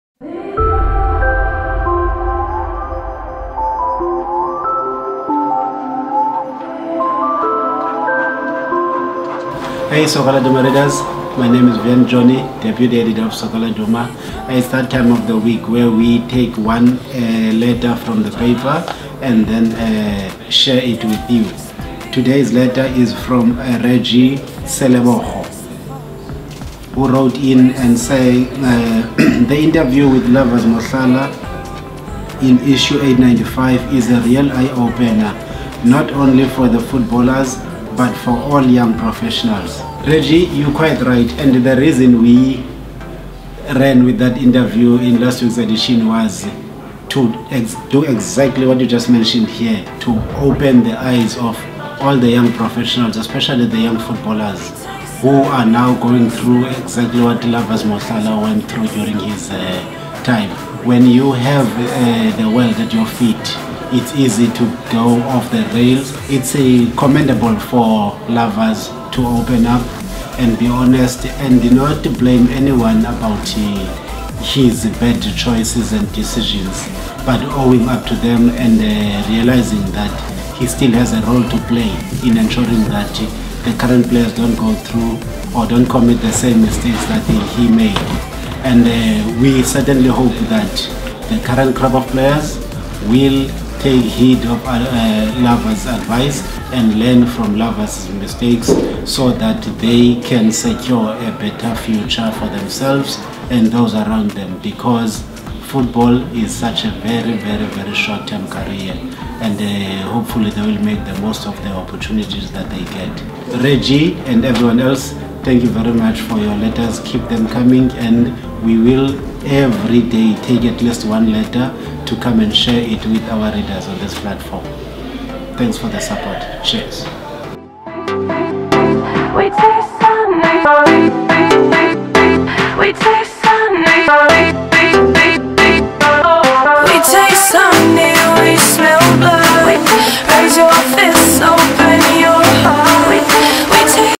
Editor Replies To Your Letters - Lovers Mohlala Gives Back
In today's Letters show, Soccer Laduma's deputy editor Vuyani Joni discusses the subject of Lovers Mohlala's interview in Soccer Laduma's 985th edition.